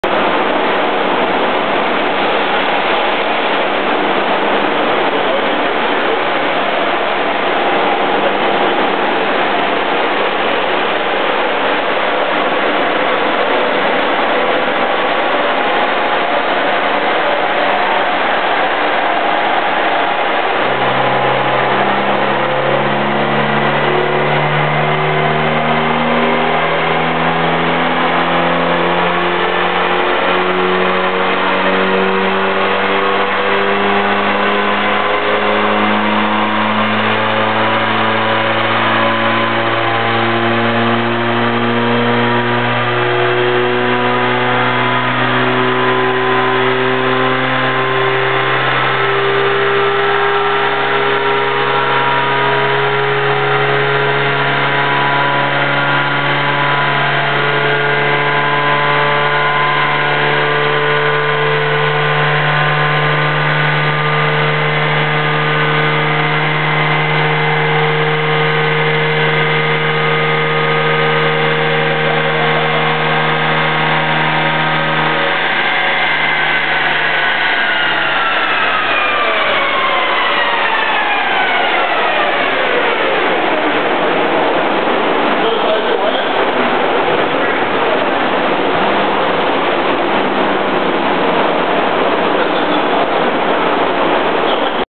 1984 Honda Accord 4th Gear Dyno Pull
Dyno'd my car at Wong's Performance Engineering in Vancouver, WA. We tried this first pull in 4th gear, which in this car is overdrive. The car failed to pull past 4,500RPM.